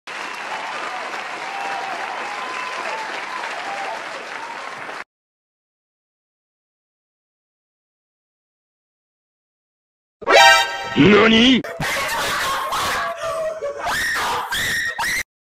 Bruh(Wtf is this...)
We legit had 302 subscribers but some a**holes decided to unsubscribe(sorry for calling them that,but I legit hit 300 subscribers which I was planning to reach by the end of the year)